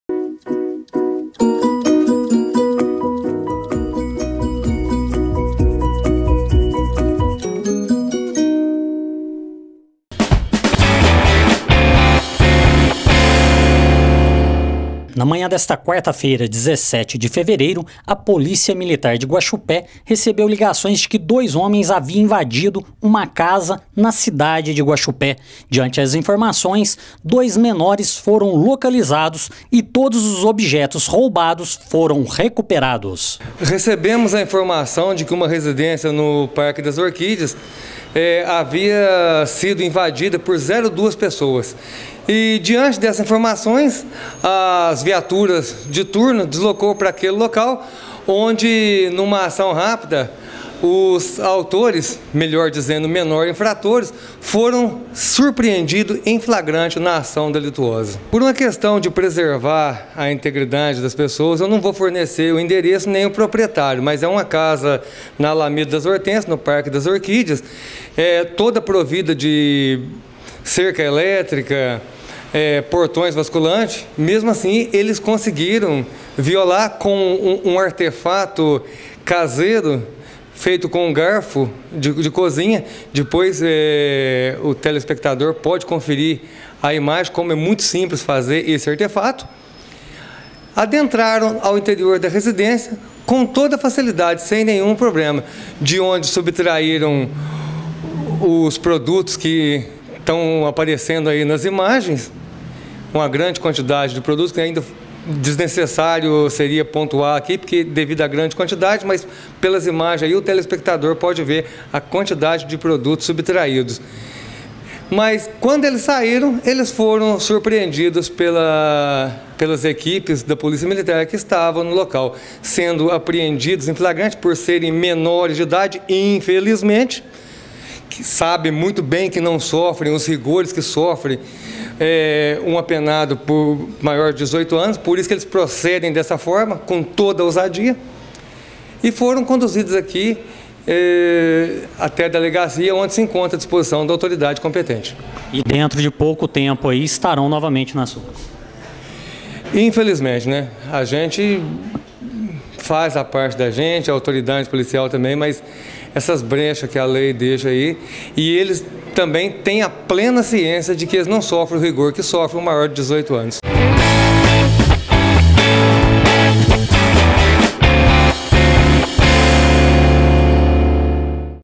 Polícia Militar apreende menores minutos depois de furtarem uma residência
Na manhã desta quarta-feira 17 de fevereiro, a Polícia Militar recebeu ligações de que uma residência estava sendo furtada, na Alameda das Hortência, no bairro Parque das Orquídeas na cidade de Guaxupé (MG). Rapidamente os policiais iniciaram o rastreamento e em poucos minutos conseguiram localizar os envolvidos e recuperar todos os objetos furtados. Os menores já tem envolvimento em assalto à mão armada, tráfico de drogas e furtos, além de outros crimes menores. Foi utilizado um garfo preparado para abrir o portão da casa.